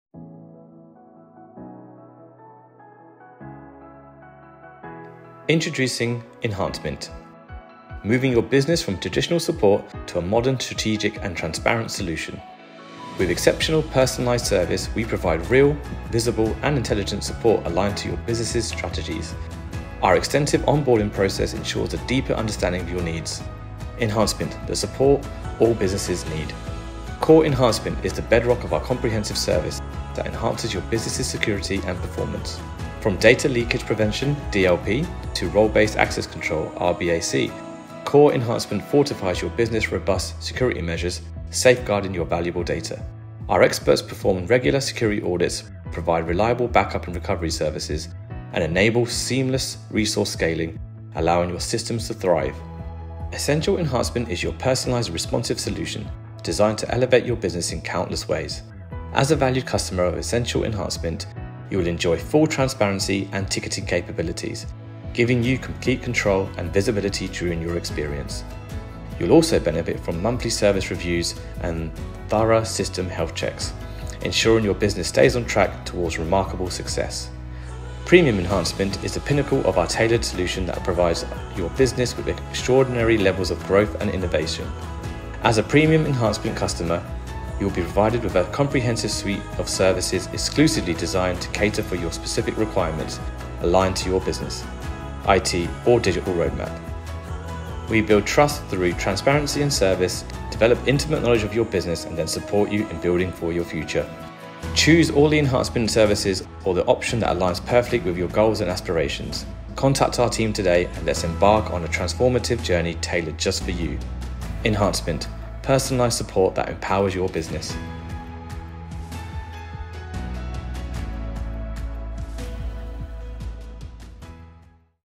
Advanced Dynamics 365 Support | ERP Support Solution | EnhanceMint
EnhanceMint is advanced Dynamics 365 support. We enhance and intensify the productivity and efficiency of your business so that your core focus is returned to your own roles, functions, and business strategies.

Now you can reach your full digital and strategic potential through innovative technology and solutions.